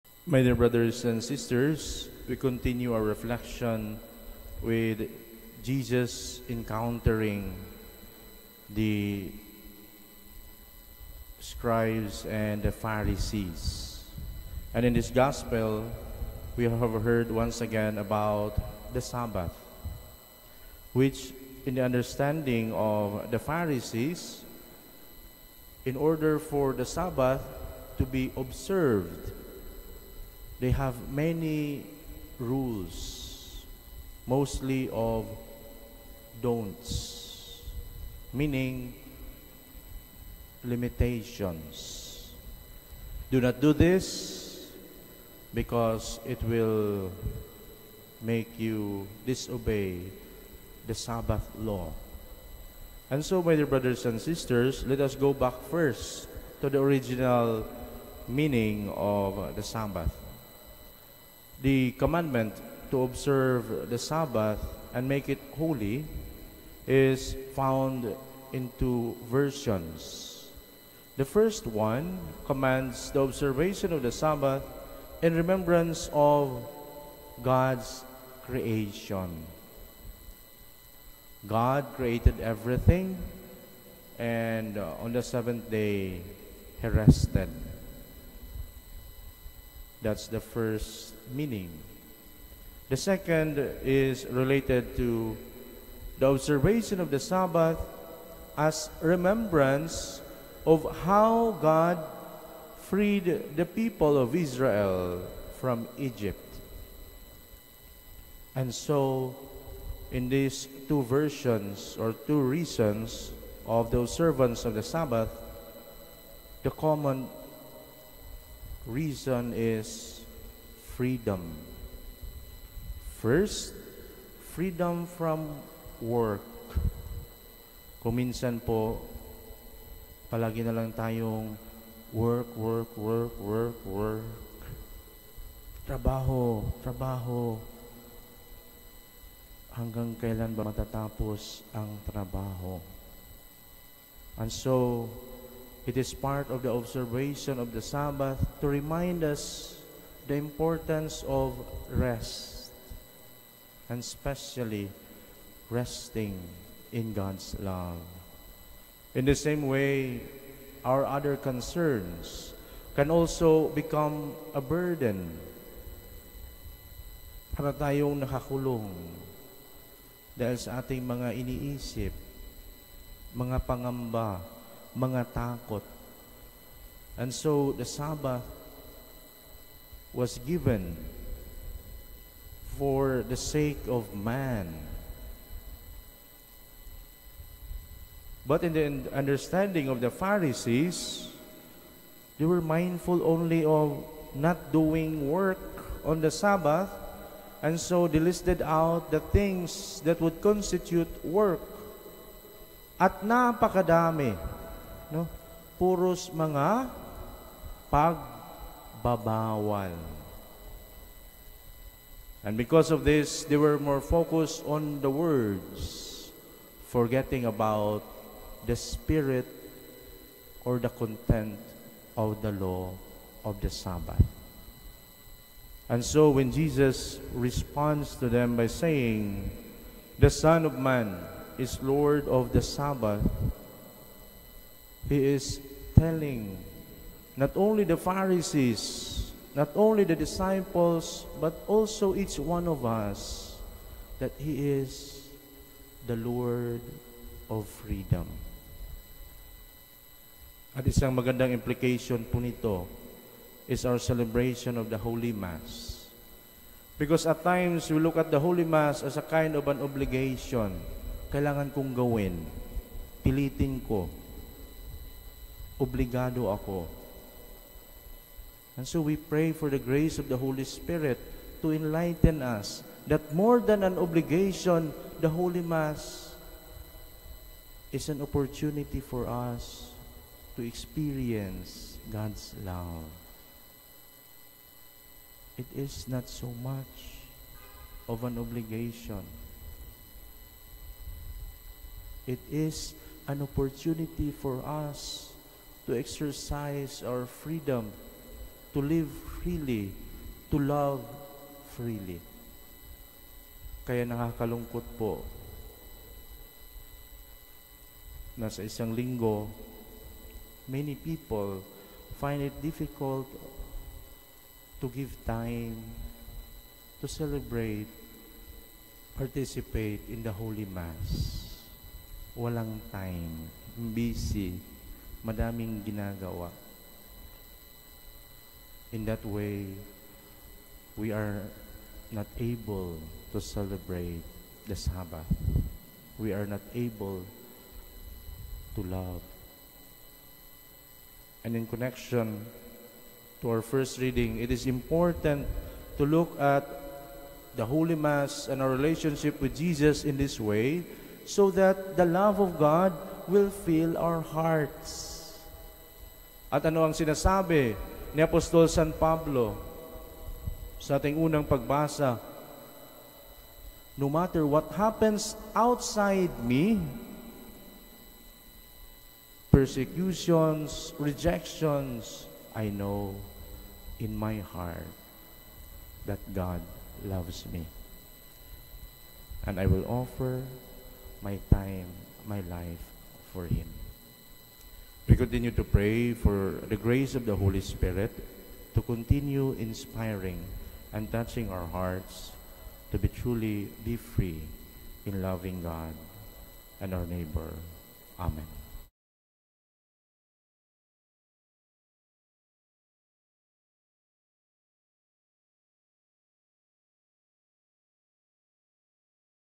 Homily Reflection of Rev. Fr. Michael Creencia, SVD
Holy Mass, 6:30am First Saturday of the Twenty-Second Week in Ordinary Time, September 07, 2024, at the Diocesan Shrine of Jesus the Divine Word at Christ the King Mission Seminary, Quezon City, Philippines.

"May the darkness of sin and night of unbelief vanish before the light of the Word and the spirit of grace, and may the heart of Jesus live in the hearts of all. Amen" (St. Arnold Janssen)